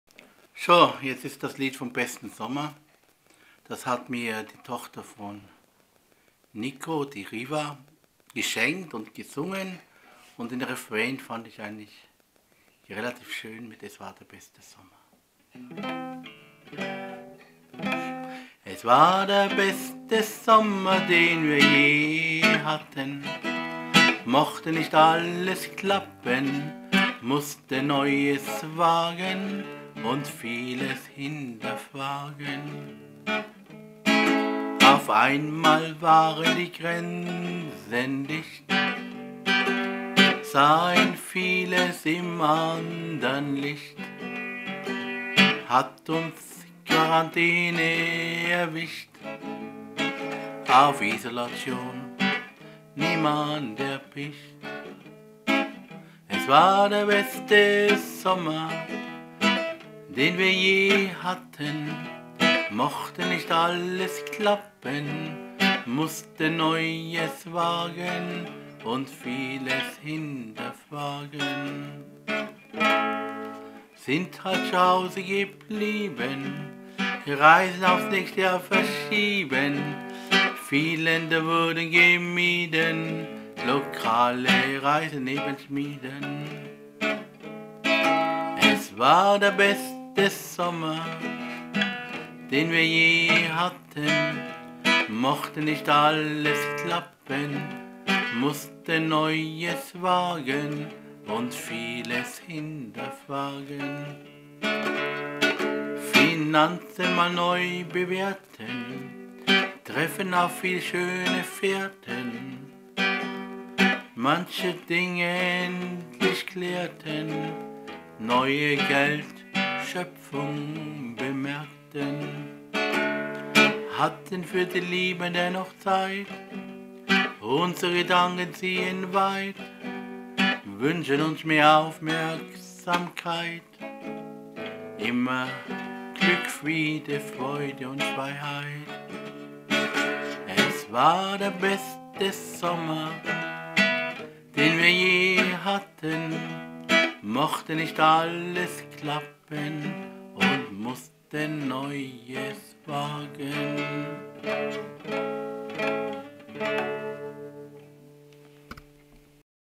Bester Sommer
Der Sommer 2020 ist für viele Kinder trotz Corona toll gewesen, die Eltern hatten mehr Zeit